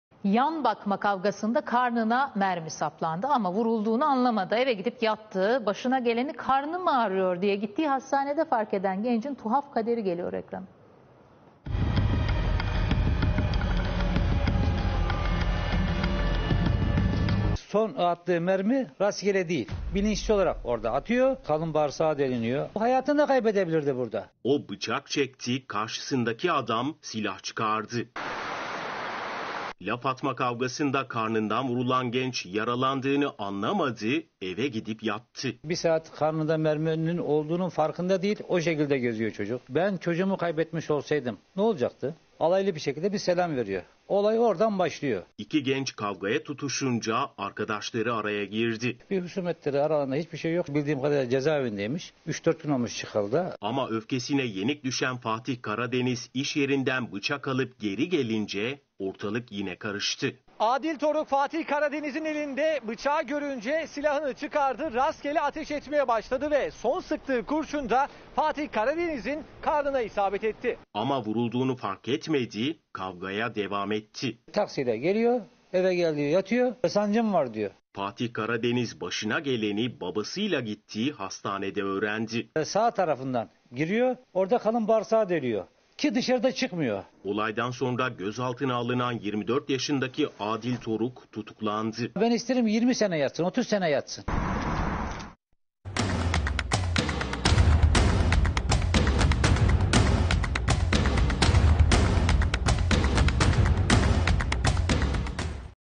Kavgada mermi yedi,1 saat sonra fark etti
YAN BAKMA KAVGASINDA KARNINA MERMİ SAPLANDI, AMA VURULDUĞUNU ANLAMADI, EVE GİDİP YATTI... BAŞINA GELENİ 'KARNIM AĞRIYOR' DİYE GİTTİĞİ HASTANEDE FARK EDEN GENCİN TUHAF KADERİ GELİYOR EKRANA...

Haber: Aziz Akova